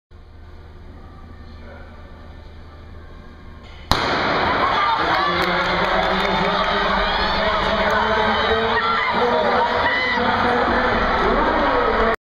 2012 Big Ten Championship 60m Final
B1G 10 Championship 60m Final
1st Place - Andrew Riley (6.62)
2nd Place - Kind Butler (6.67)
3rd Place - Brandon Stryganek (6.76)
4th Place - Raheem Mostert (6.77)
5th Place - Devantre Whitelow (6.80)
6th Place - Troy Doris (6.80)
7th Place - Tim Thompson (6.88)
8th Place - Brandon Blackwell (7.15)